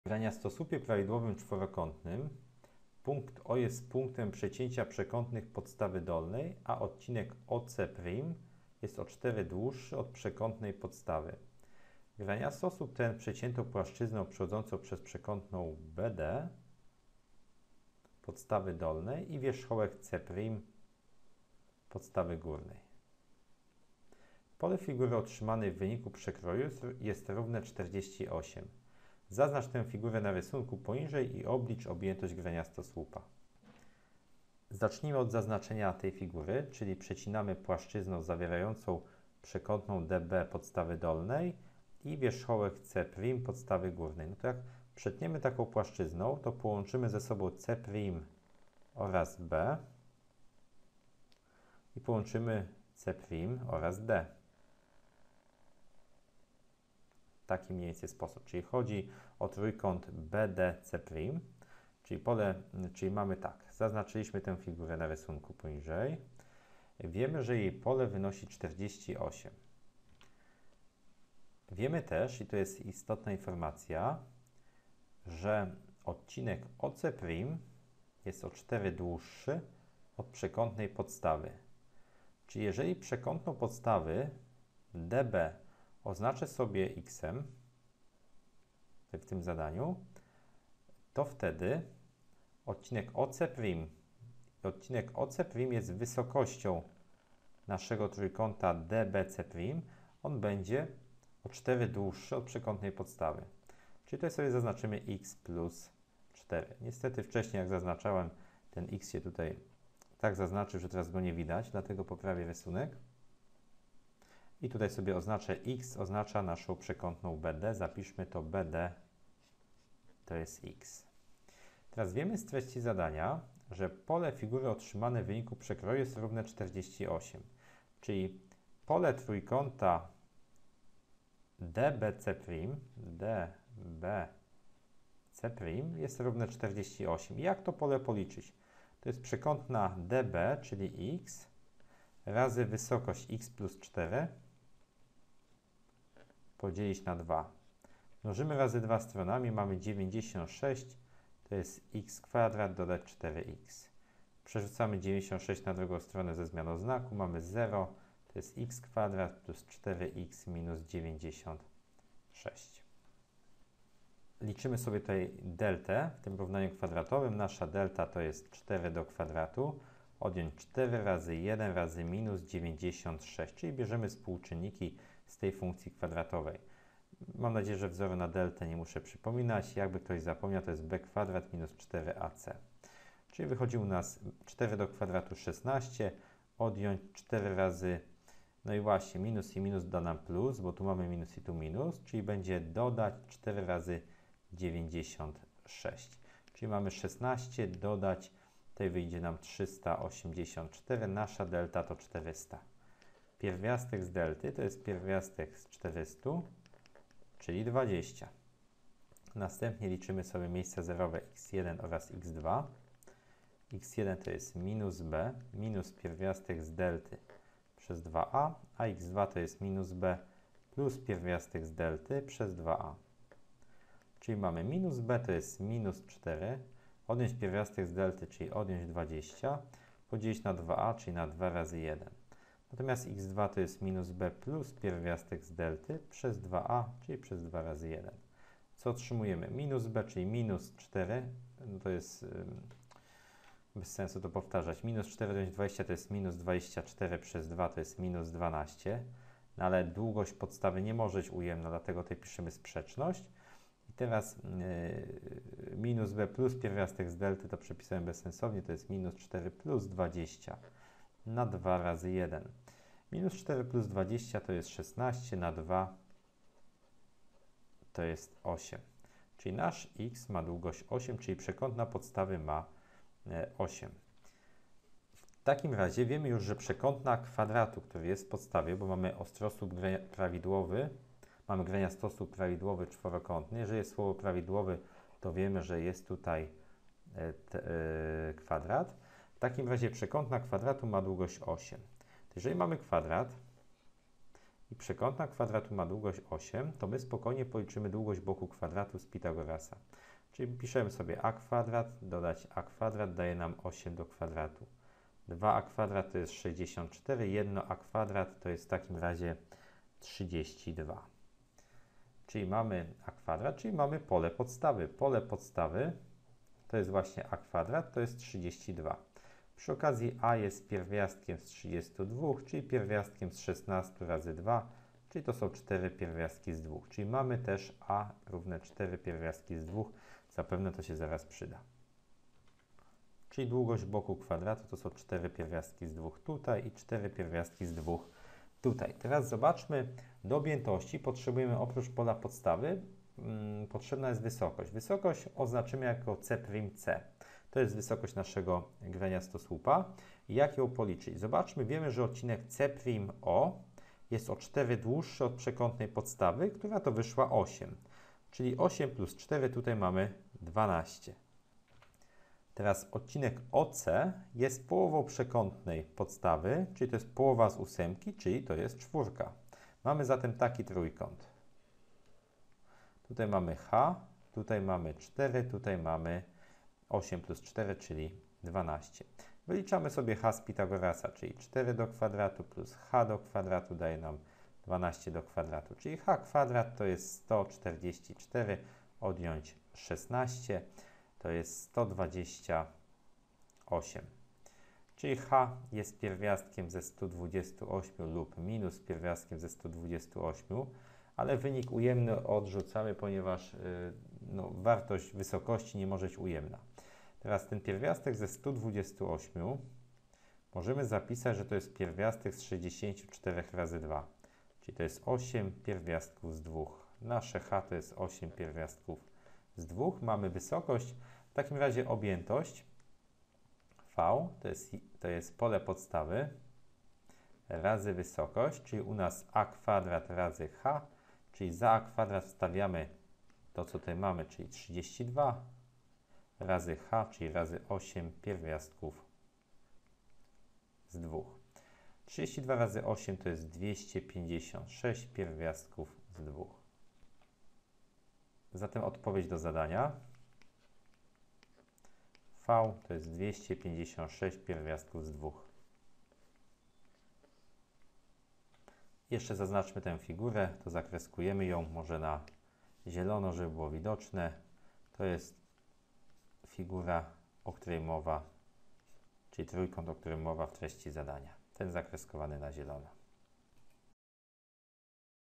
33 W graniastosłupie prawidłowym czworokątnym zobacz rysunek poniżej punkt O jest punktem przecięcia
W graniastosłupie prawidłowym czworokątnym (zobacz rysunek poniżej) punkt O jest punktem przecięcia przekątnych podstawy dolnej, a odcinek OC′ jest o 4 dłuższy od przekątnej podstawy. Graniastosłup ten przecięto płaszczyzną przechodzącą przez przekątną BD podstawy dolnej i wierzchołek C′ podstawy górnej. Pole figury otrzymanej w wyniku przekroju jest równe 48. Zaznacz tę figurę na rysunku poniżej i oblicz objętość graniastosłupa.